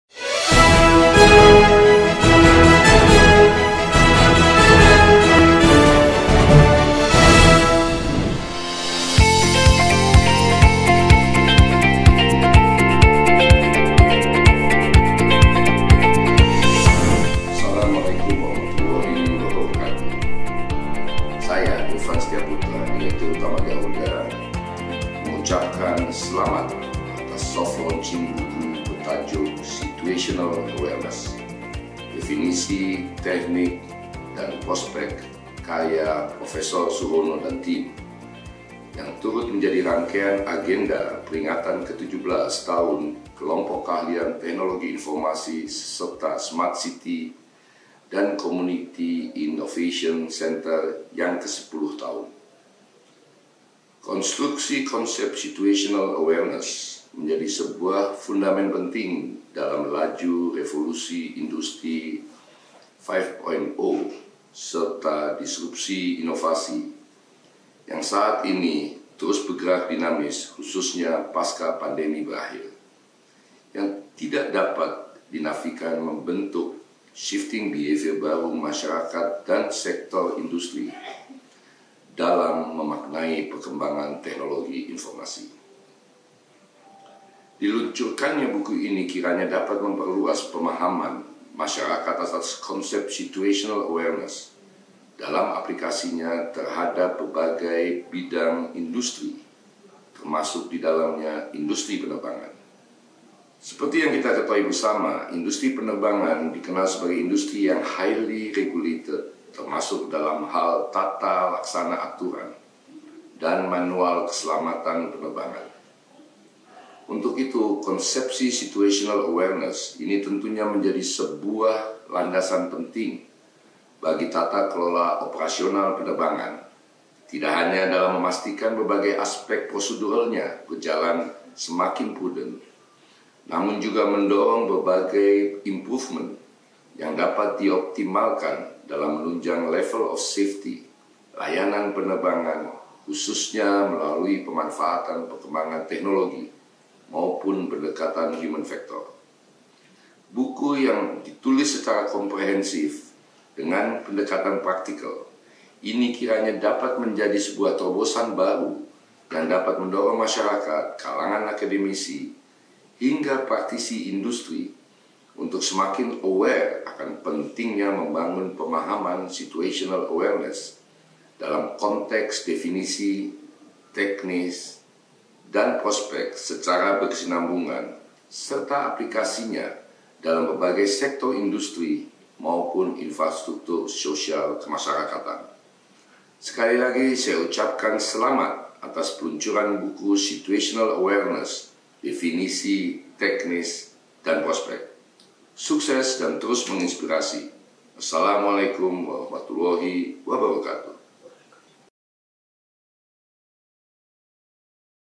Irfan Setiaputra (Direktur Utama Garuda Indonesia) - Pandangan & Komentar Buku Situational Awareness
Pada kegiatan Open House, Pameran Inovasi SCCIC dan KKTI, Talkshow dan Diskusi Buku dalam rangka “17 Tahun KKTI, 10 Tahun  SCCIC: Transformasi Digital dan Kesadaran Situasional menuju Smart Indonesia” yang diadakan pada tanggal 4 Desember 2023 di Bandung, Irfan Setiaputra (Direktur Utama Garuda Indonesia) berpartisipasi dalam Diskusi buku. Beliau memberikan pandangan dan Komentar Buku: Situational Awareness.